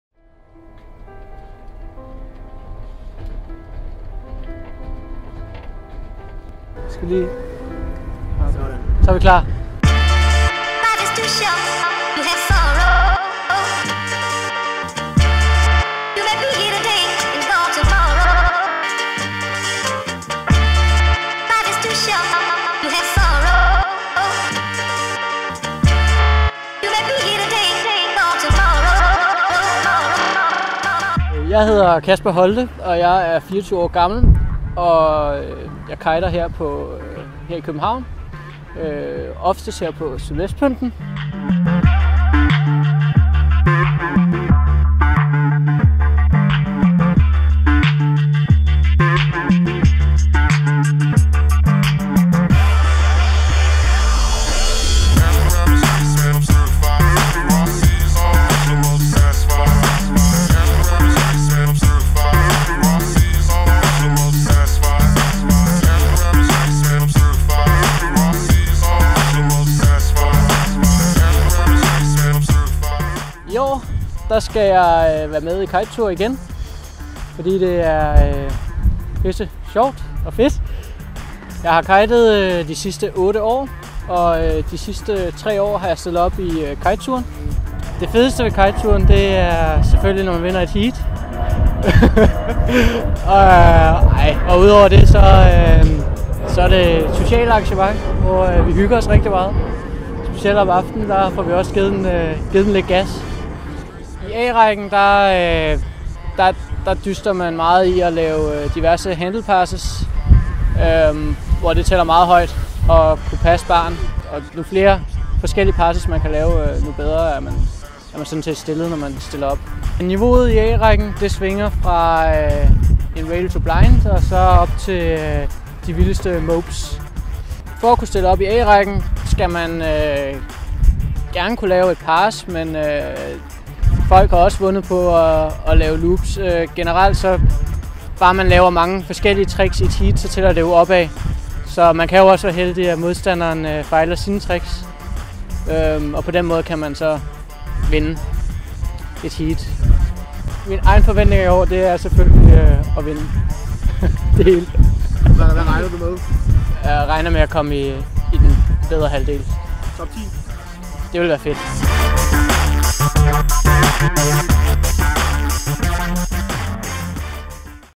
kitetour teaser 2012 sønderborg
Film med Casper holte om kitetour i sønderborg 2012